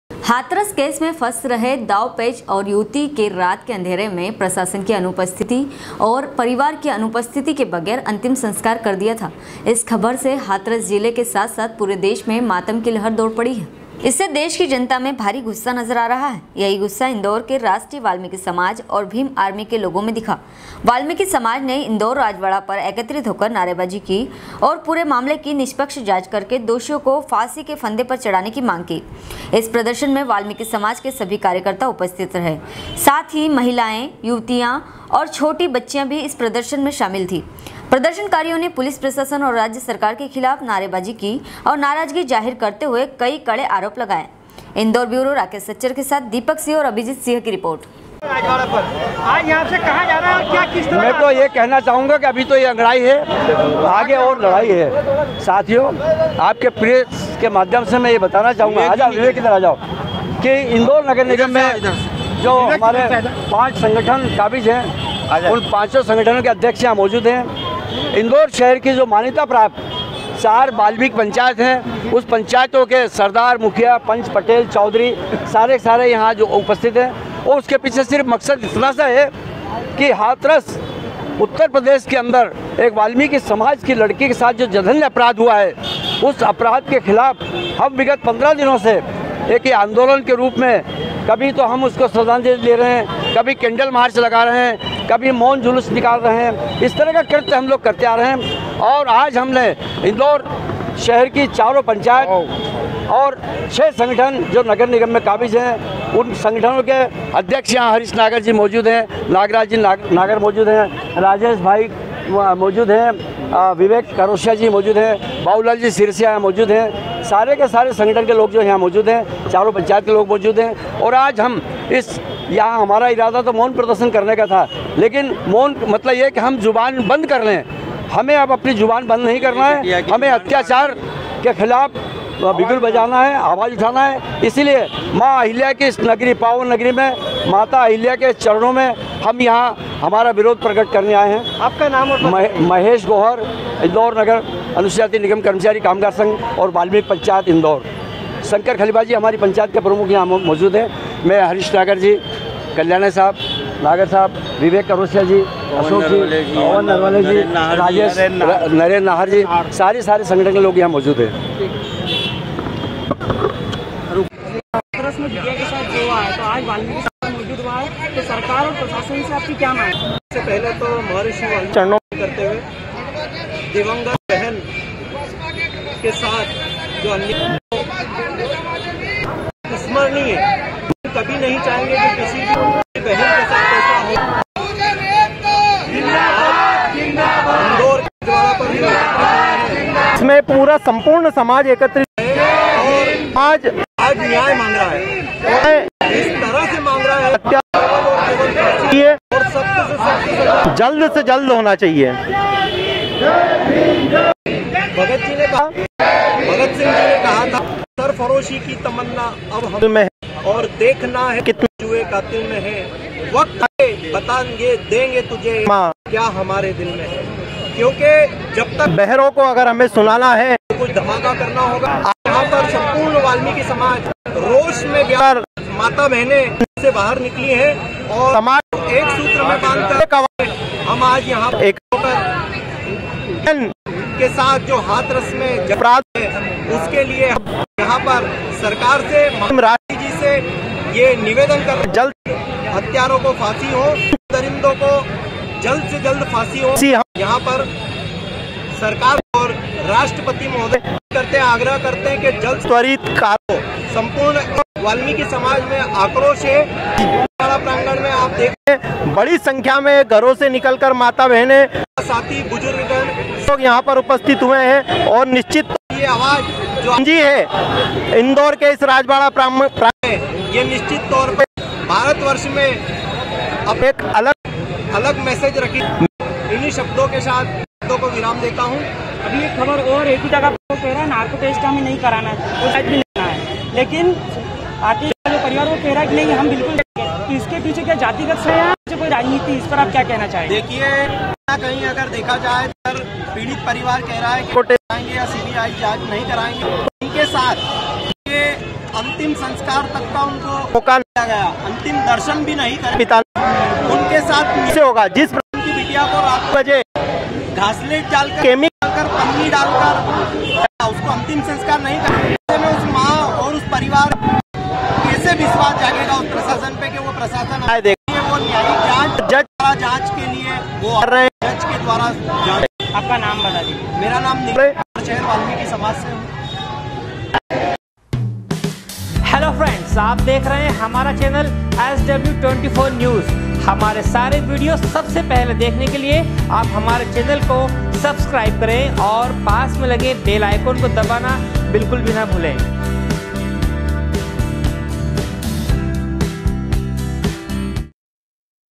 SW 24 News Madhyapradesh
हाथरस केस में फंस रहे दाव पेच और युवती के रात के अंधेरे में प्रशासन की उपस्थिति और परिवार की अनुमति के बगैर अंतिम संस्कार कर दिया था इस खबर से हाथरस जिले के साथ-साथ पूरे देश में मातम की लहर दौड़ पड़ी है। इंदौर ब्यूरो राकेश सच्चर के साथ दीपक सिंह और अभिजीत सिंह की रिपोर्ट